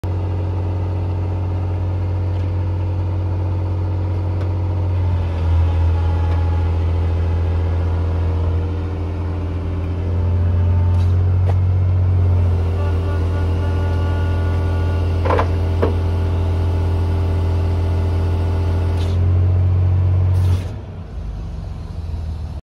Volvo BM 5350 med risrede - 91 926536 2/4
Volvo BM 5350 med risrede - 91, Djurmo, Auktionsdatum: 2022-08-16, 28852 tim

Volvo BM 5350 with rice nest - 91, Djurmo, AuctionDate: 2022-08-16, 28852 hours